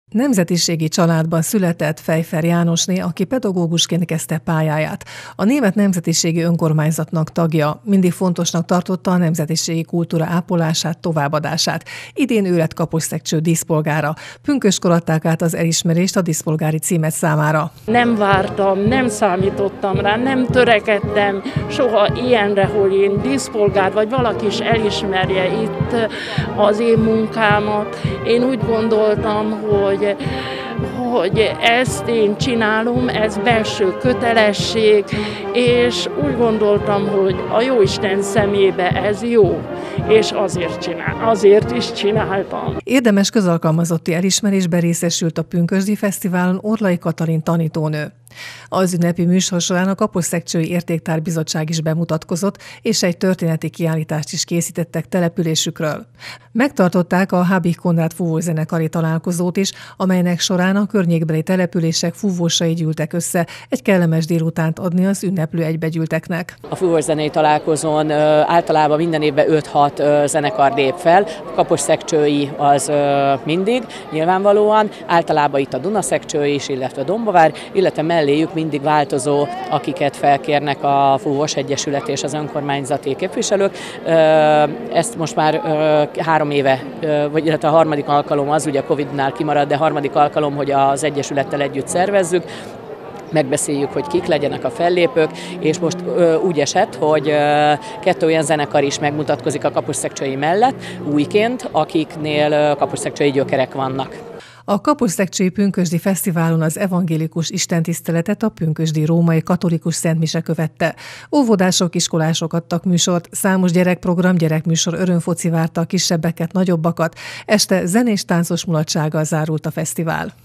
Pünkösdi Fesztivált rendeztek Kaposszekcsőn - fúvószenekari találkozó és kitüntető címek átadása
Pünkösdi Fesztivált rendeztek Kaposszekcsőn. A program keretében megtartották a Habich Konrád Fúvószenekari Találkozót is.
Az egész napos ünnep alkalmával bemutatkozott a Értéktár Bizottság és kitüntető címeket is átadtak. Számos gyerekprogram, valamint zenés mulatság várta a pünkösdölőket.